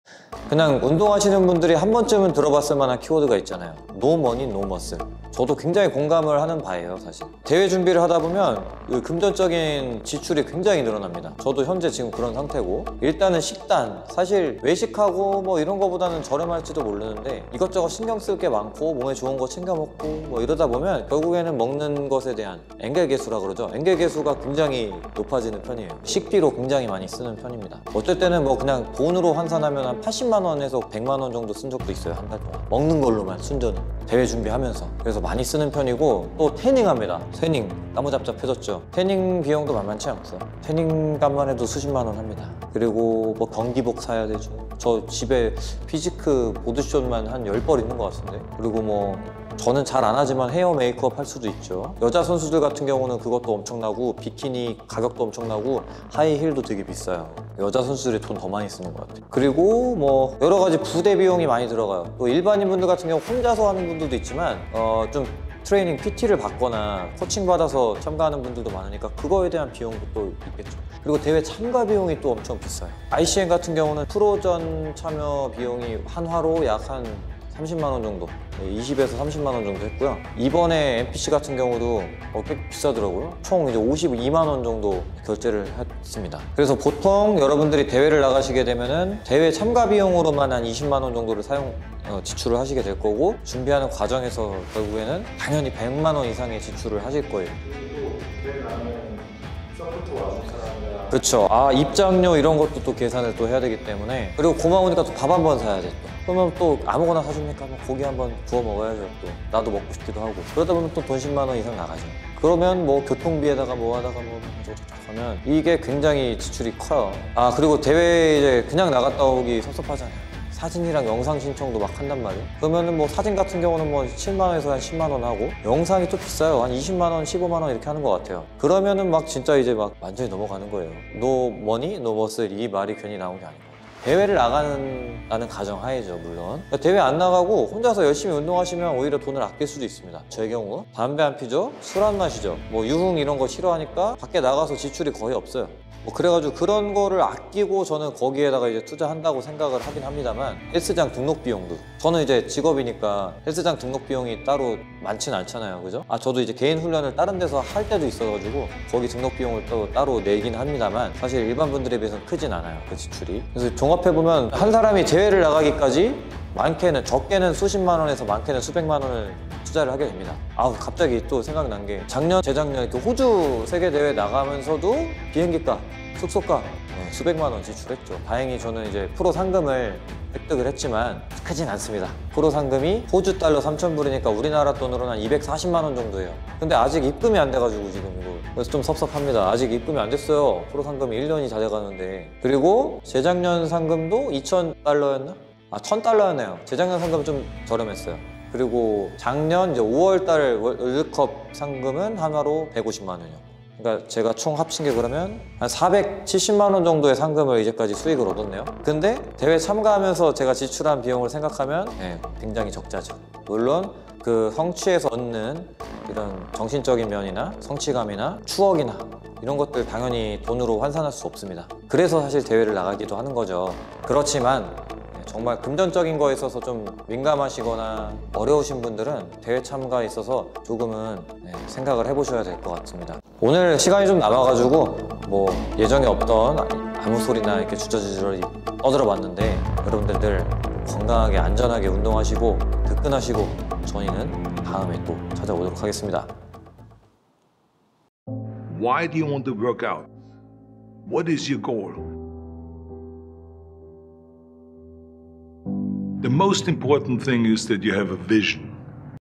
돈이 줄줄 새는 대회 준비 - 노머니 노머슬
보디빌딩, 피트니스 대회 준비에 드는 비용들에 대해 얘기해봤습니다.
태닝, 식비, 서포터, PT비, 참가비 등..

[랭킹닭컴 앰버서더 채널]
← 추천아이디 'PUSH' 자동입력 링크

상기 링크에서의 구매는 채널 운영에 보탬이 됩니다.